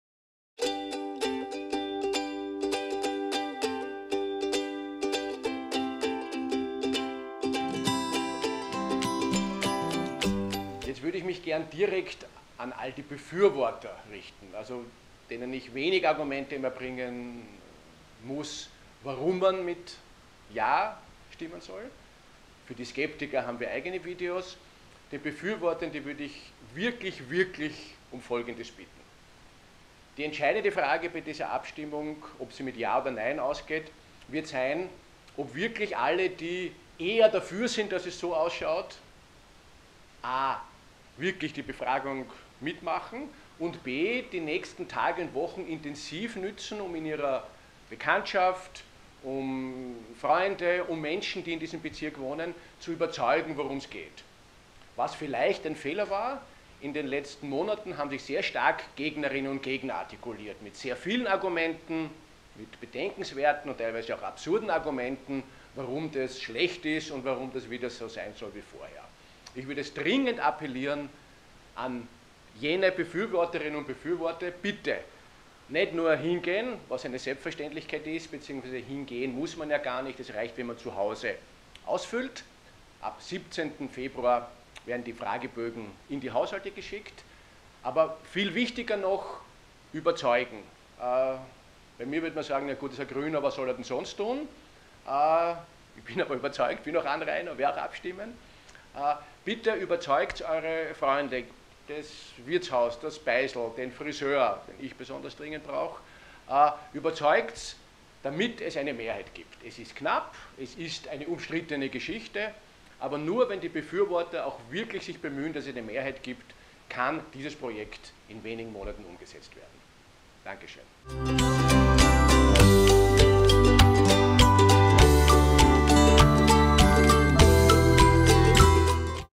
Die Mahü Sprechstunde mit Christoph Chorherr Teil 2: Überzeugen, Überzeugen, Überzeugen!
Christoph Chorherr erklärt in mehreren Videos die wichtigsten Fragen und Argumente zur Verkehrsberuhigung der Mariahilfer Strasse. Hier der zweite Teil.